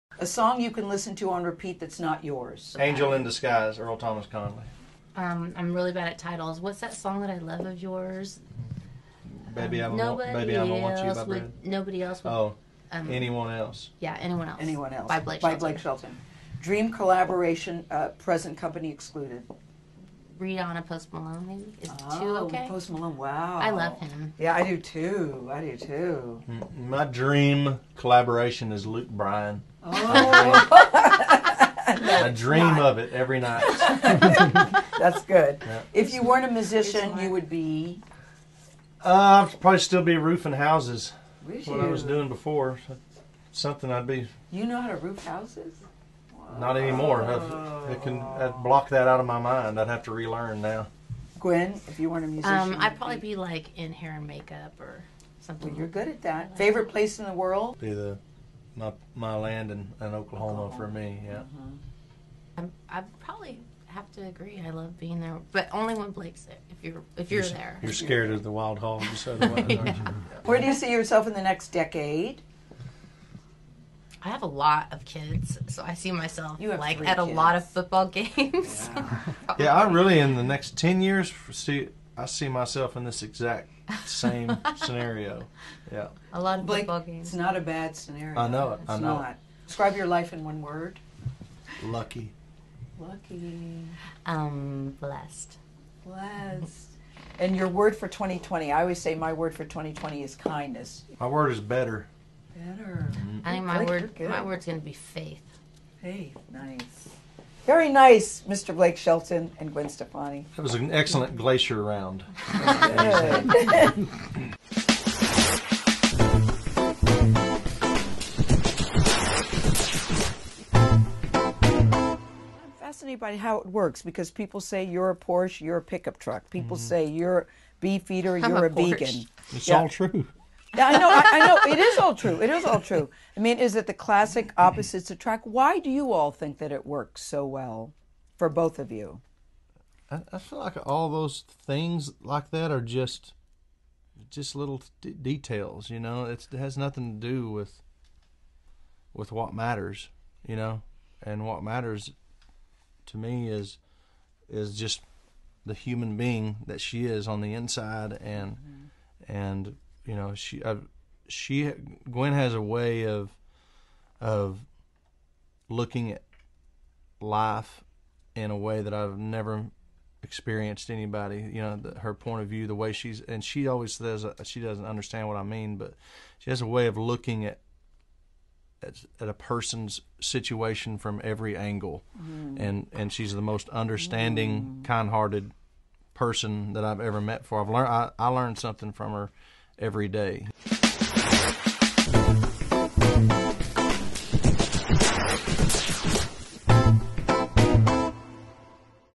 Blake Shelton & Gwen Stefani Grammy Interview
Gayle King interviewing  Blake Shelton and Gwen Stefani. A few days before the Grammy’s.